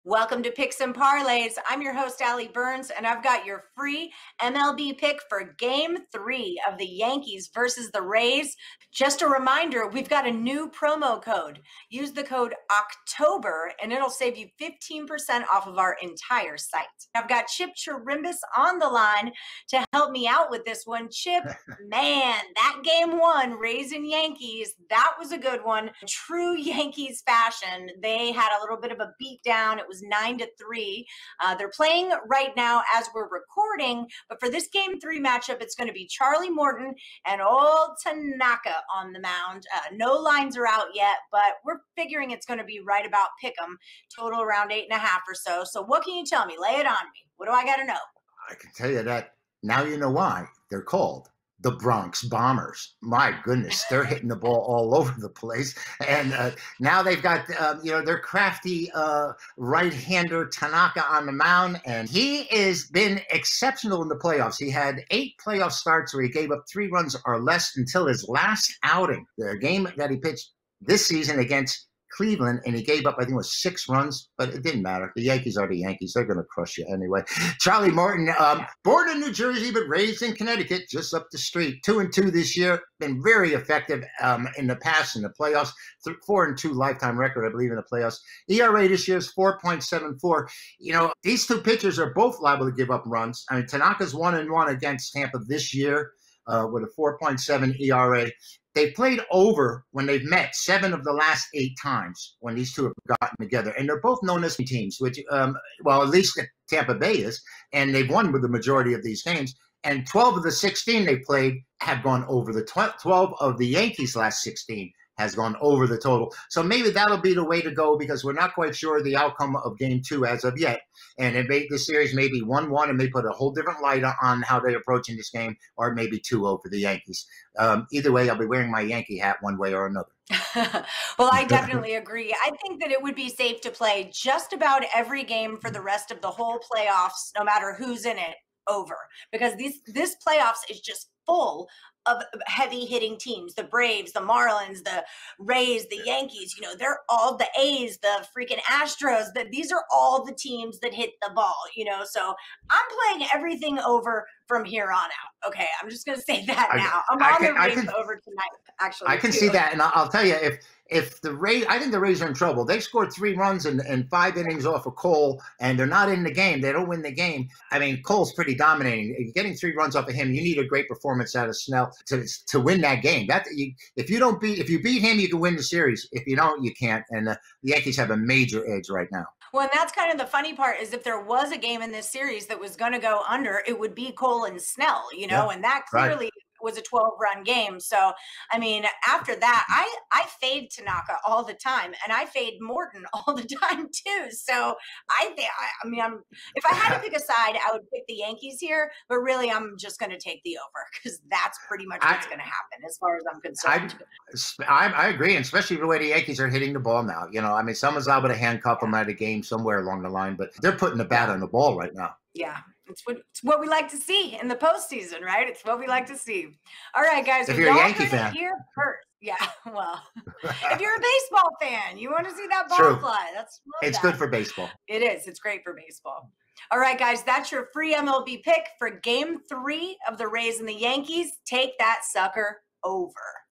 Rays vs Yankees - Game 3 - Wednesday 10/7/20 - MLB Picks & Predictions | Picks & Parlays
Tampa Bay Rays vs New York Yankees - Game 3 - Wednesday 10/7/20 - MLB Picks & Predictions

The MLB Postseason is here and we have all your MLB betting needs covered. Today our free betting pick is in the Tampa Bay Rays vs New York Yankees on Wednesday October 7, 2020 and we have your expert Betting Pick from Picks & Parlays experts Ali Burns and Chip Chirimbes.